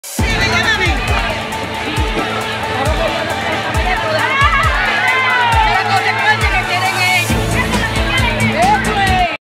Mujeres se desmayaron en colas para sacarse la cedula en Venezuela.
Estado Zulia, las personas salen desmayadas por hacer largas colas en las oficinas  del SAIME para intentar sacarse las cédulas de identidad.

Ésta sede corresponde a Zumaque en el municipio San Francisco, según relatos de los ahí  presentes algunas personas salieron golpeados.